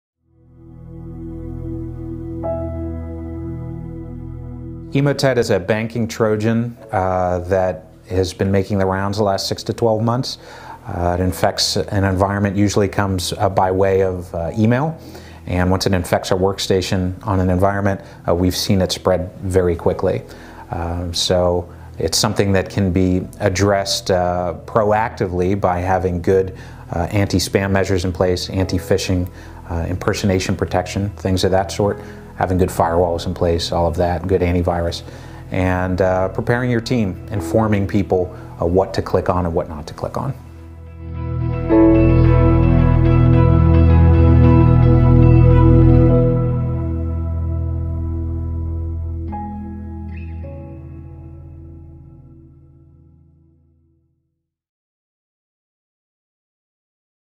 What you should know about the Emotet virus | JENLOR
Watch Frank Schlatterer, JENLOR’s Chief Technology Officer explain one of the latest malicious attacks in the current threat landscape: the Emotet Virus.